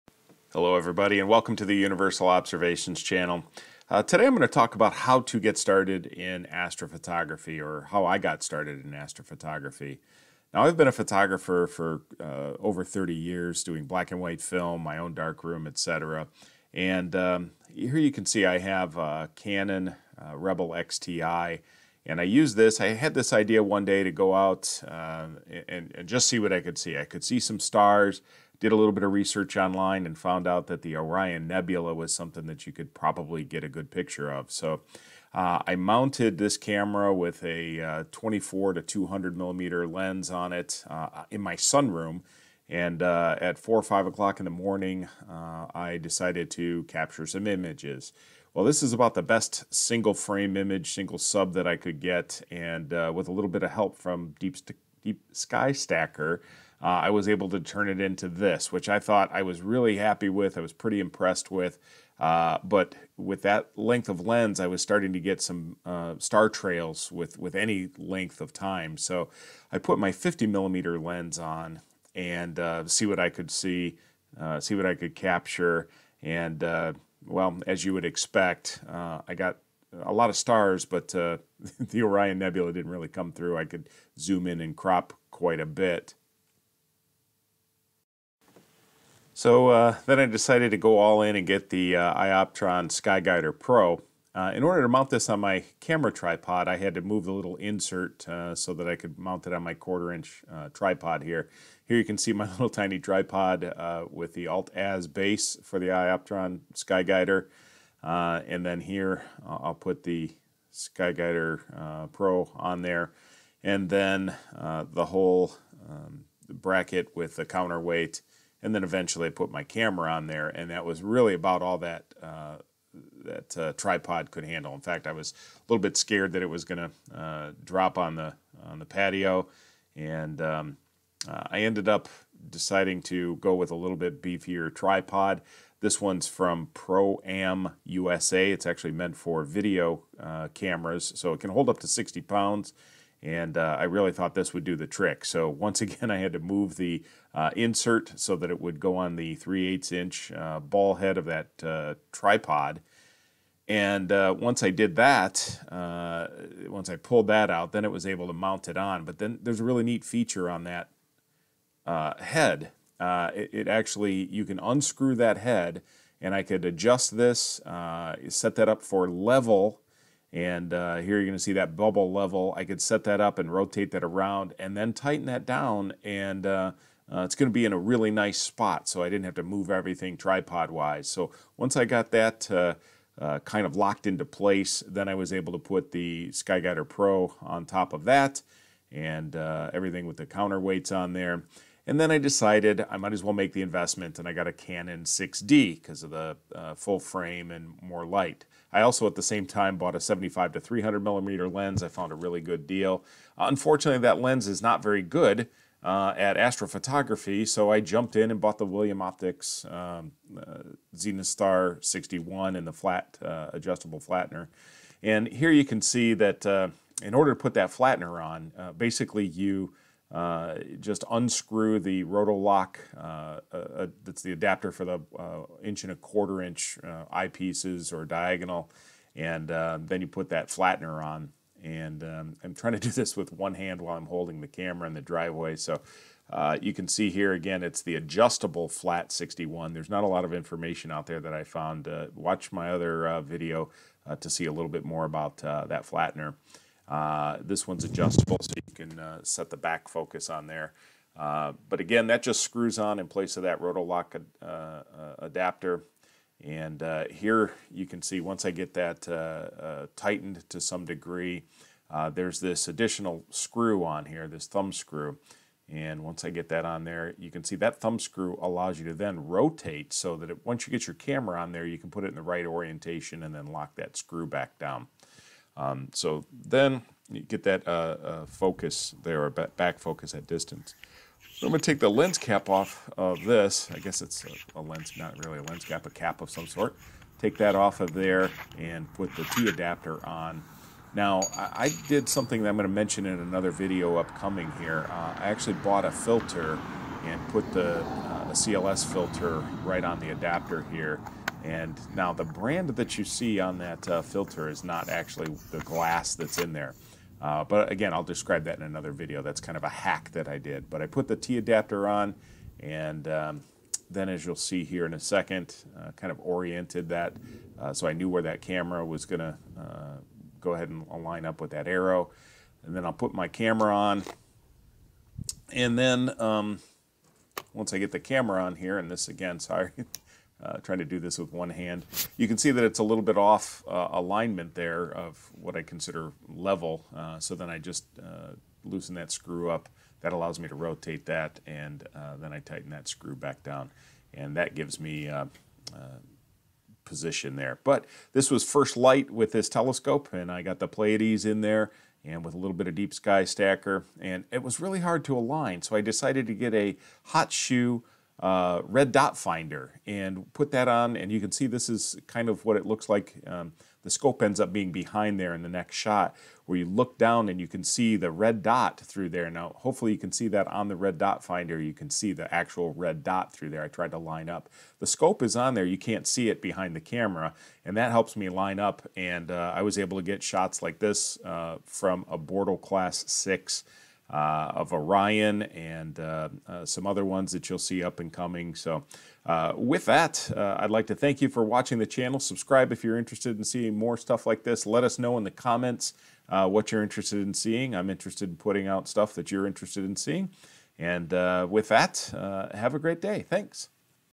How To Get Started In Astrophotography
The steps I took to get started in Astrophotography. Started with a Canon Rebel xTi and 24mm-200mm lens, tried the 50mm lens. Then purchased the iOptron Skyguider Pro, and larger tripod. Next bought the Canon 6D full frame for its low noise performance. Then stepped up to the William Optics ZenithStar 61 and Adjustable Flat 61 field flattener and a red dot finder. Shown are some of my first light images of the Pleiades (M45) and the Orion Nebula (42)

More information on UniversalObservations.com

Links to items shown in the video:

iOptron Skyguider Pro
ProAm USA Tripod
Astromania 1X40RD Reflex Red Green Dot Sight